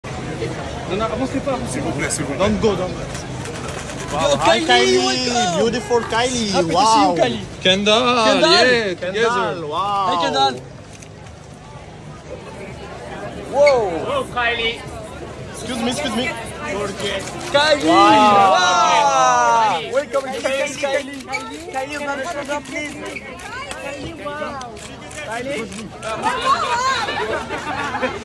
Kylie & Kendall Jenner Rock A Fur Coat & Ostrich Skin Dress To Dinner At Siena Restaurant In Paris
KylieJenner & KendallJenner Rock A FurCoat & Ostrich Skin Dress To Dinner At The Siena Restaurant During FashionWeek In Paris, France - TheHollywoodFix.net
[Video & Imagery Supplied By SplashNews.com]
©️ (Copyright TheHollywoodFix)